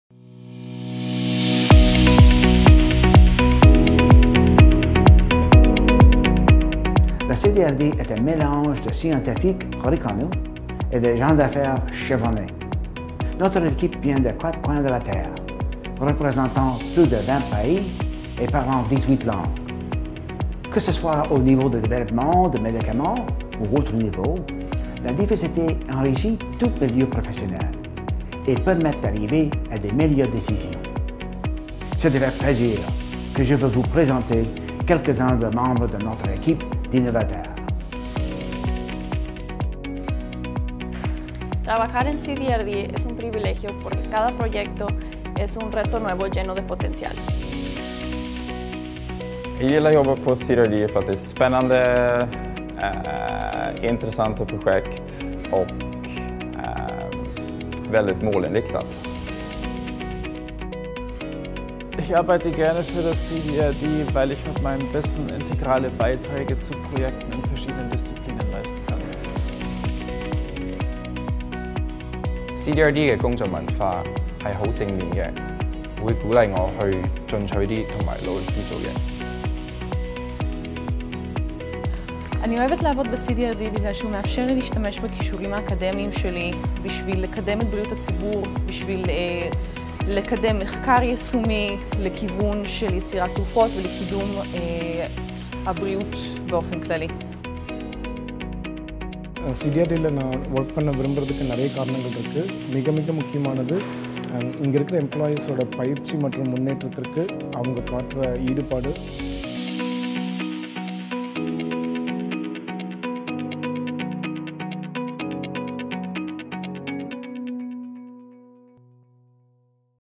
Diversity is a Strength for CDRD
Whether it be in drug development or beyond, diversity enriches any professional setting and results in better decision-making, so it gives us great pride to introduce you to just a few of our diverse team of innovators!